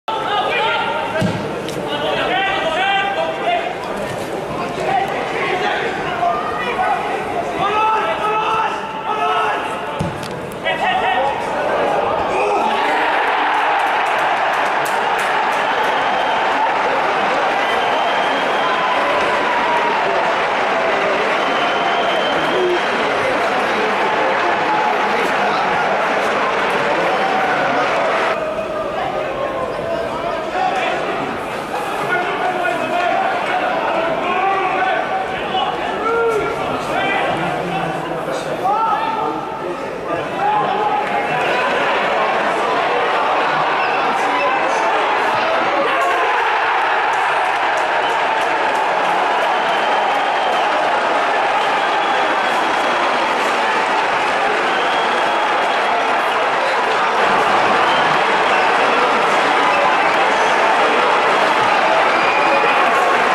Pars win keeps pressure on league leaders
Airdrieonians 0-2 Dunfermline Athletic - Ladbrokes League 1

Dunfermline remain just a point behind leaders Ayr after extending their unbeaten league run to eight games. Michael Moffat and Shaun Byrne got the goals at the Excelsior Stadium as the Pars notched up a sixth straight clean sheet.